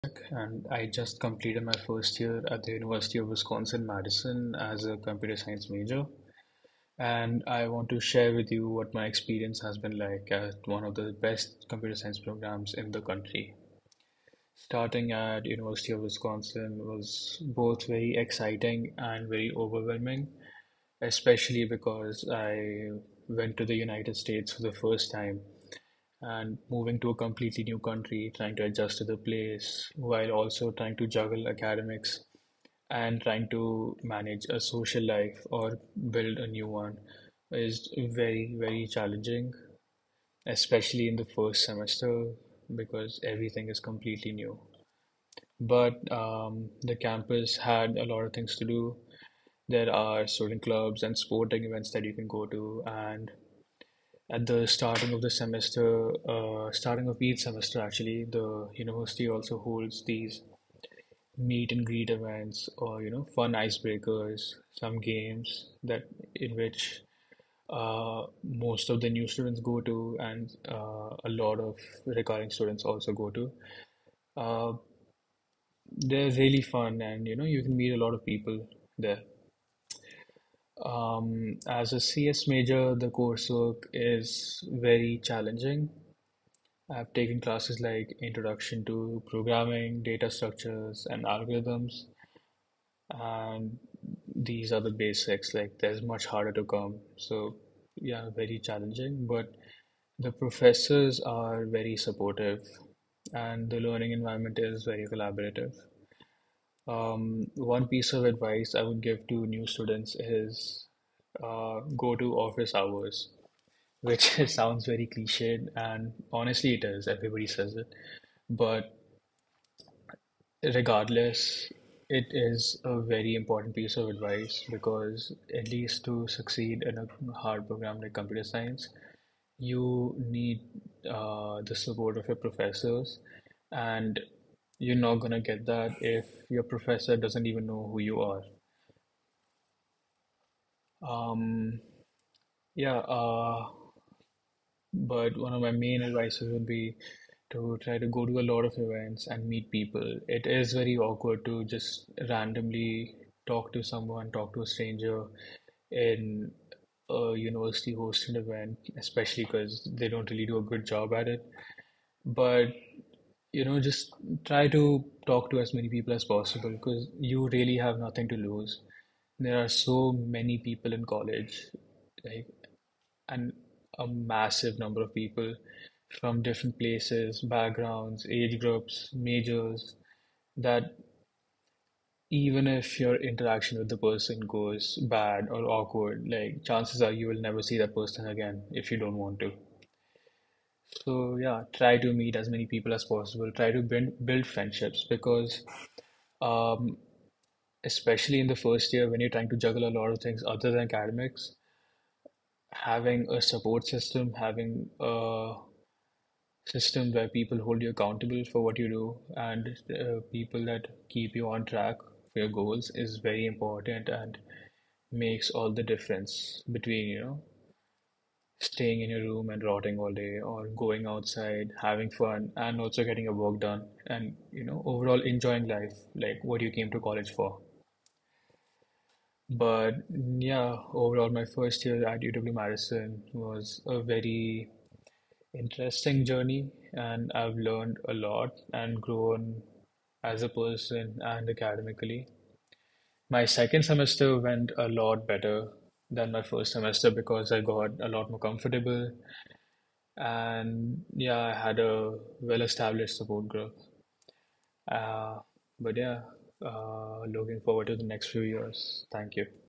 Studying Computer Science at UW Madison, advice for newcomers
The University of Wisconsin-Madison's computer science program is well-established, boasting the first ever CS PhD graduate in the US (1965). Renowned for its research, it offers BS, MS, and PhD degrees with specializations in AI, graphics, security, and more.
Award-winning faculty fosters a collaborative environment where students tackle real-world problems through project-oriented courses, preparing them for top careers or further graduate studies.